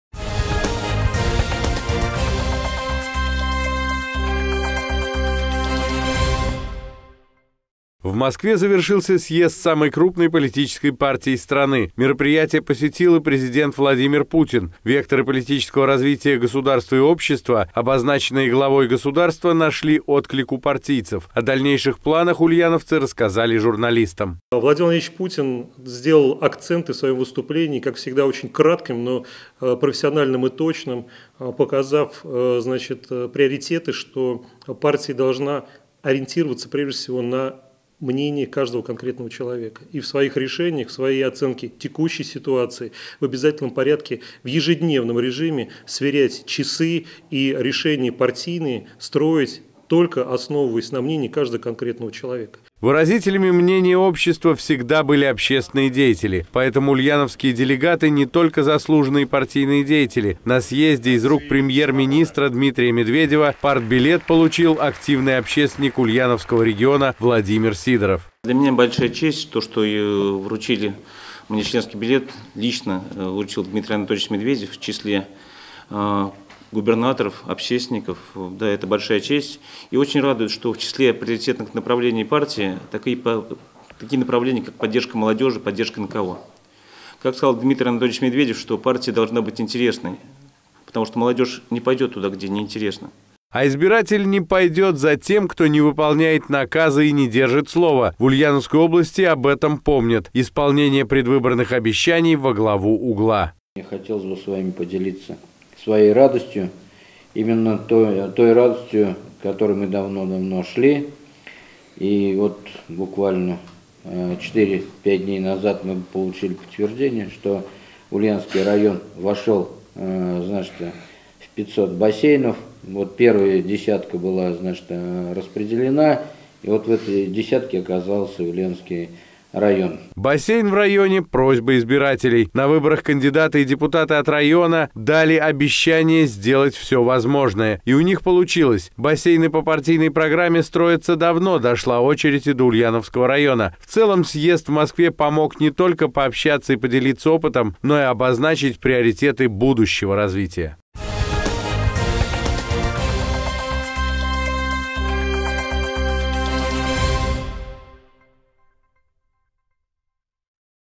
В Ульяновской области подвели итоги партийной работы
В Ульяновском отделении партии «Единая Россия» подвели итоги партийной работы в регионе и России за год. На вопросы журналистов отвечали делегаты всероссийского съезда, который недавно прошел в Москве.